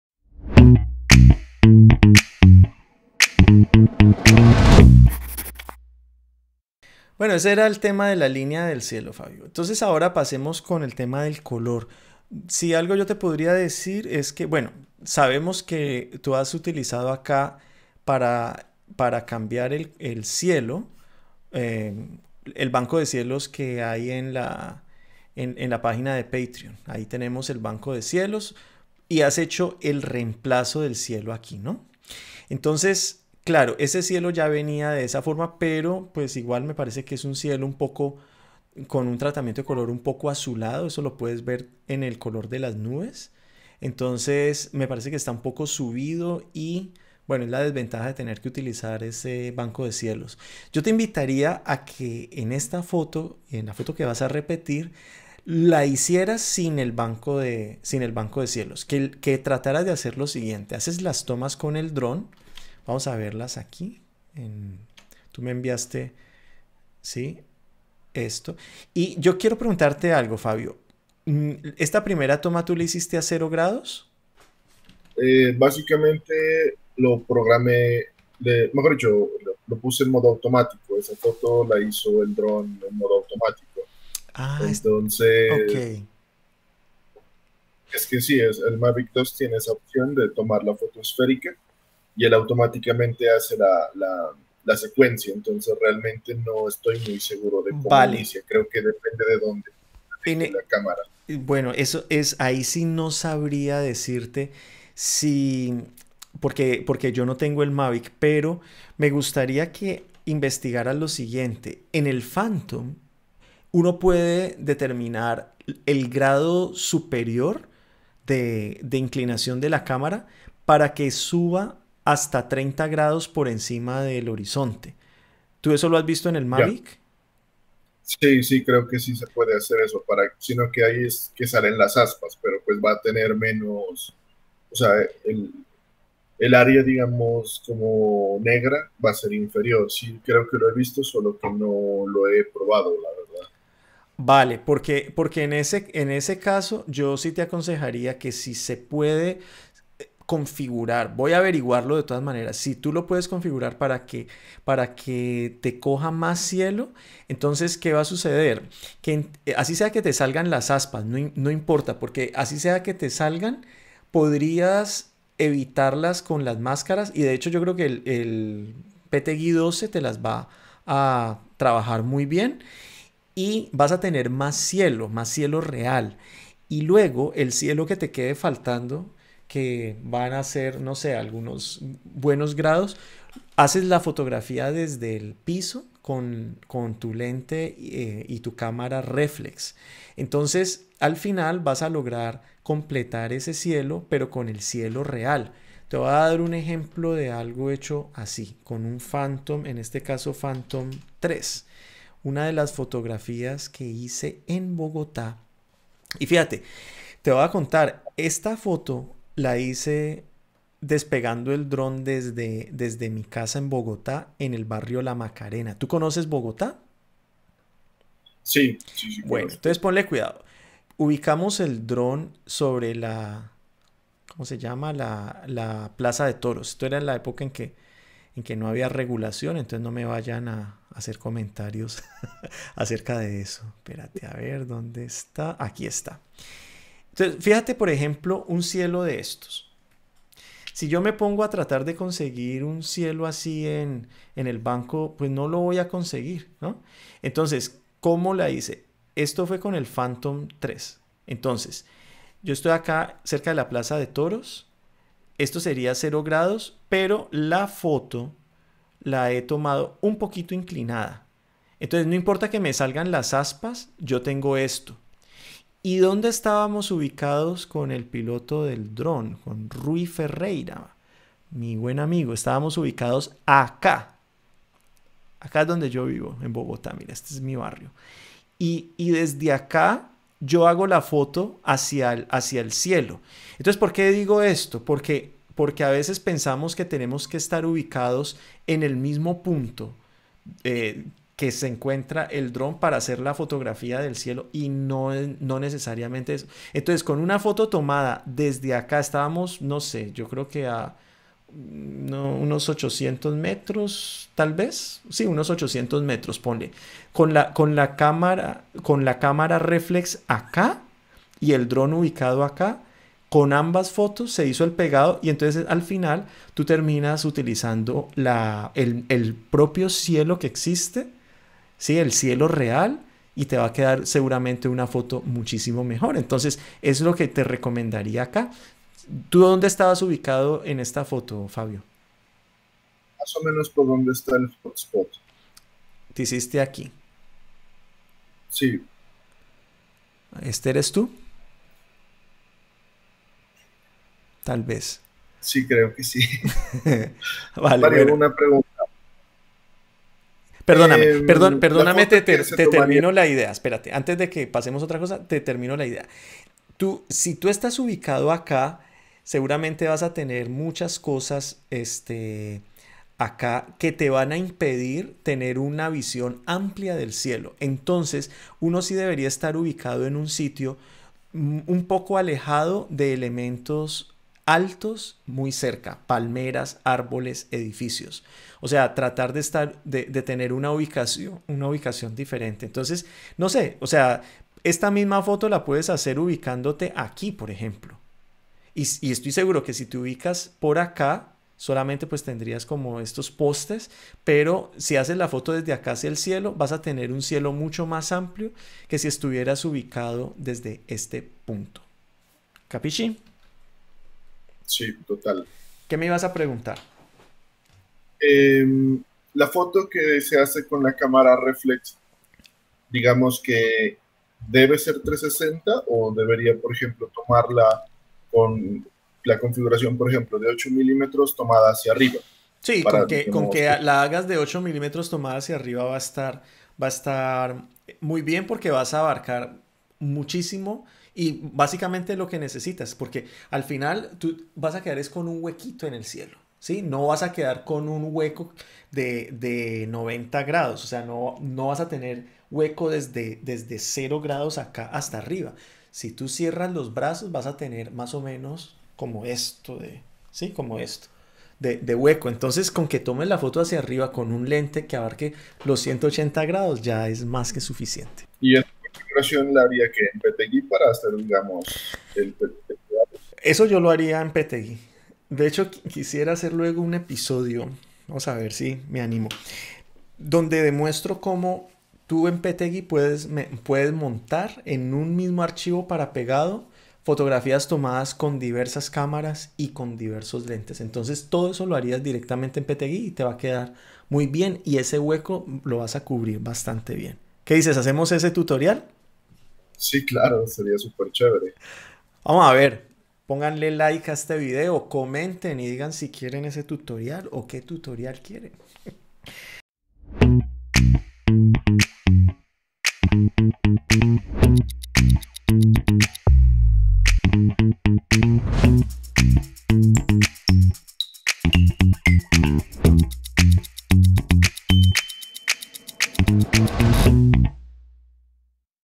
Foto aérea 360 con dron: ¿usar el banco de cielos o el cielo real? ¿Cómo? #75
Si haces fotos aéreas 360 con dron, no podrás fotografiar la totalidad del cielo, ya que la cámara está bajo el dron. ¿Qué solemos hacer? En la mayoría de casos usar el banco de cielos que he puesto a disposición en el área de Patronos de este canal o en el foro de Kamaradas.
Aunque esta es una solución rápida, no siempre resulta tan conveniente. Debes hacer coincidir dos cosas: a) La posición del Sol y b) La nubosidad. Así que no siempre será posible que coincida a la perfección, lo que hará que tu foto tenga un montaje fallido.
En esta crítica fotográfica a Fabio Montenegro hablamos de la posibilidad de tomar la foto real ampliando las posibilidades de giro del Mavic Pro (o DJI  Phantom) y completando la toma desde el piso, para luego ensamblar todo. Expongo algunos ejemplos en los que he usado el cielo real en fotografía esférica 360 tomada con dron.